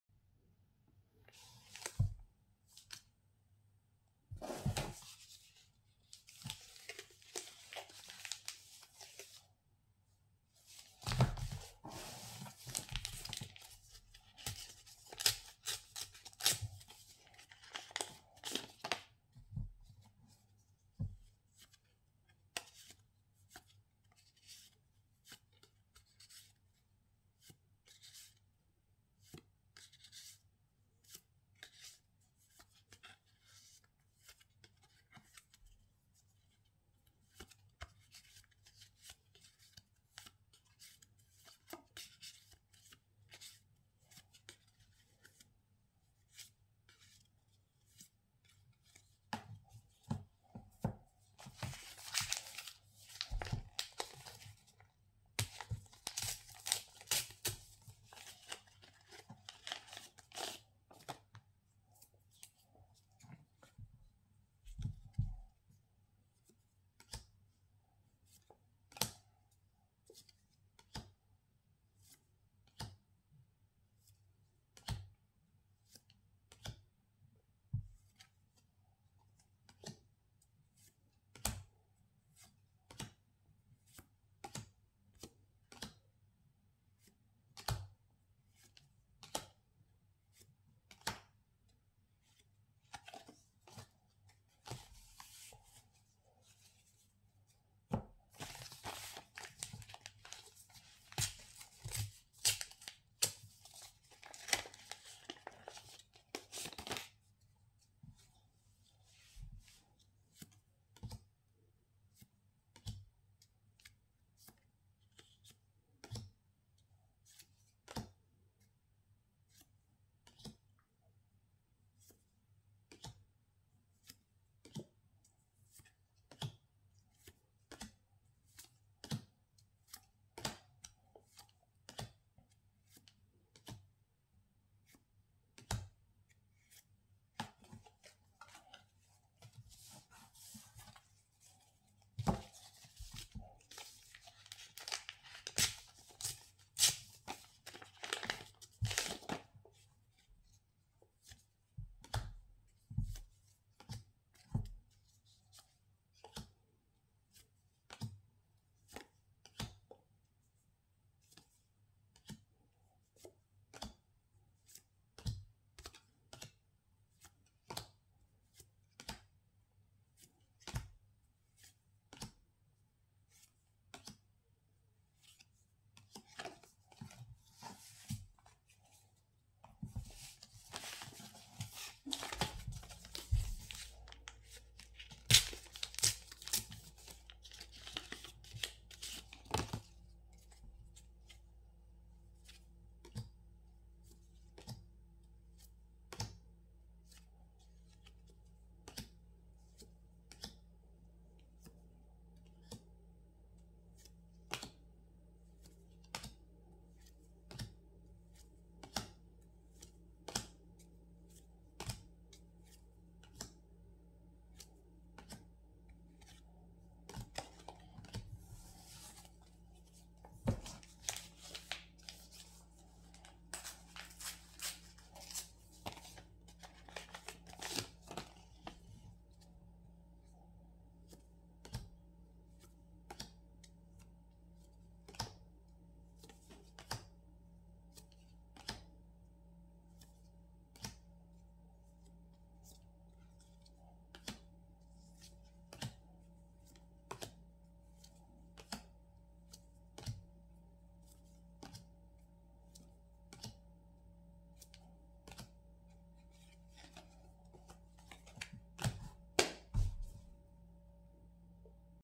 FAB : UPR In Japan 13-18/24
it's been hectic in Japan. gonna skipped a day, and can't get the two days' break done before sunset so me in my hotel room. opening pack 13 to 18 of a box of Uprising.